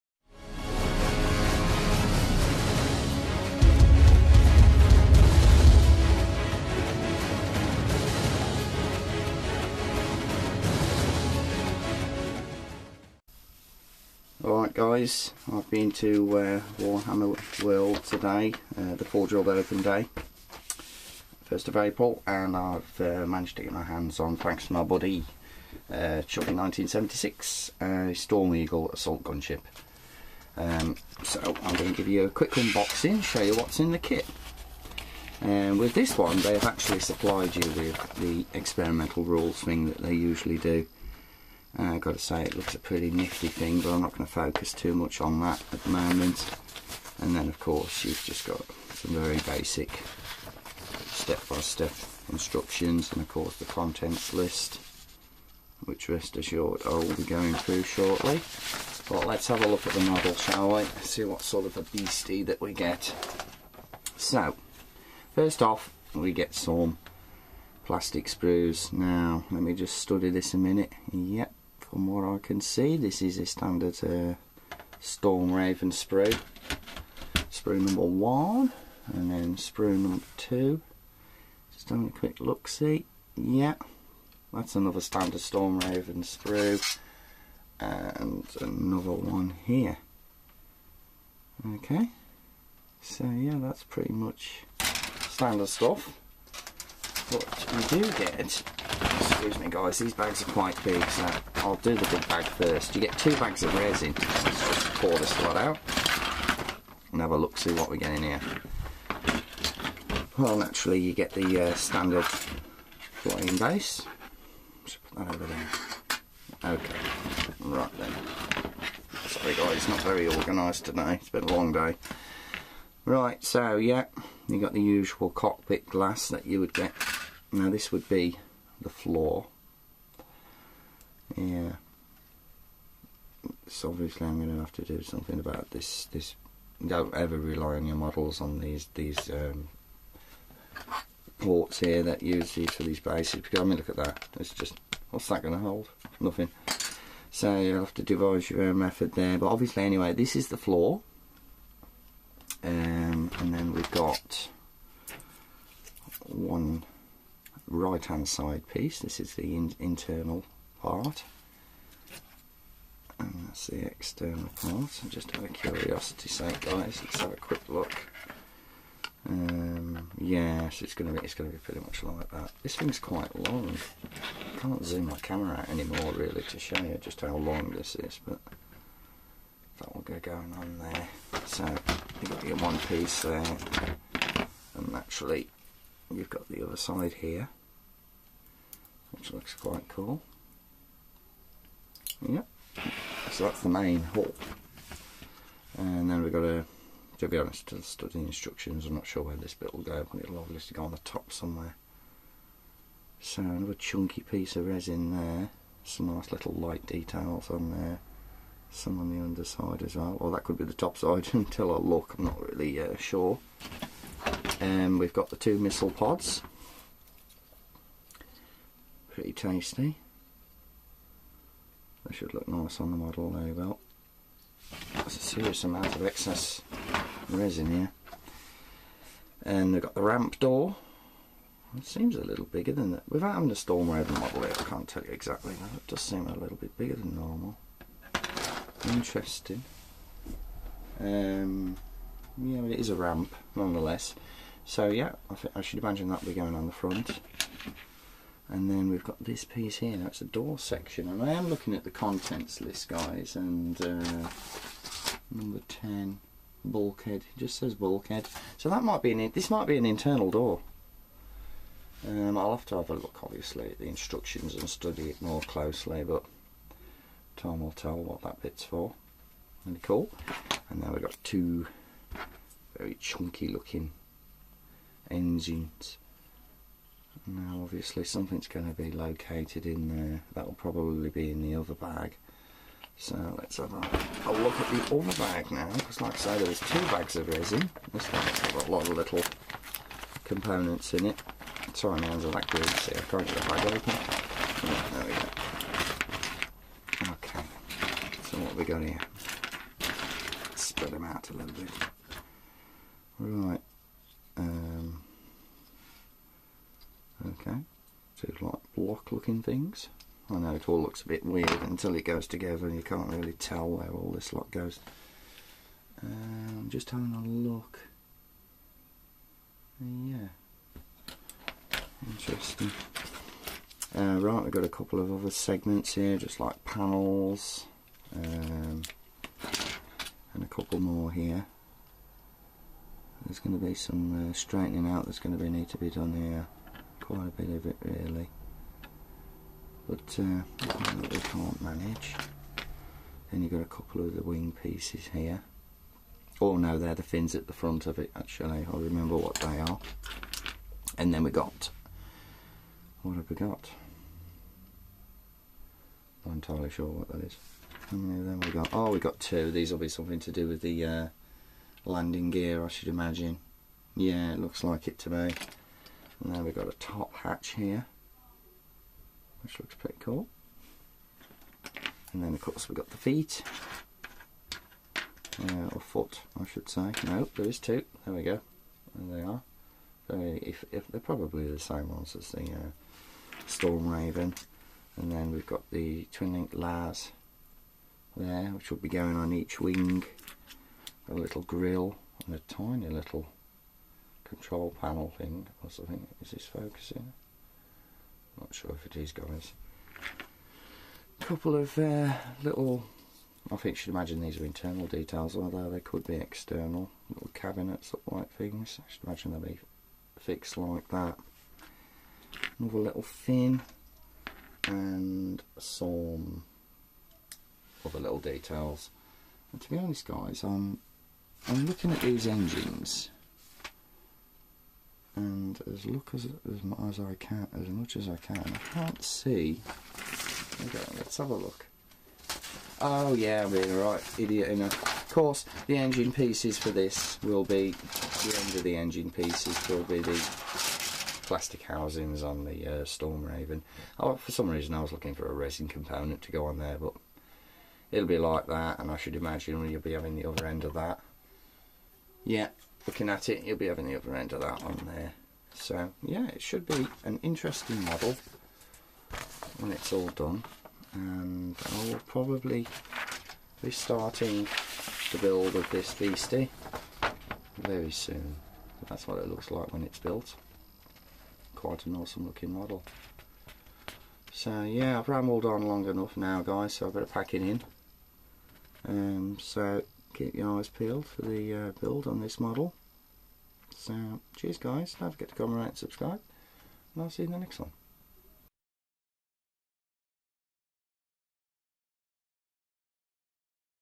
Whats in the bag? Open day pre-release Forge World Storm Eagle Assault Gunship
Just taking a look at the contents of the Storm Eagle Gunship from Forge World's open day April 1st 2012.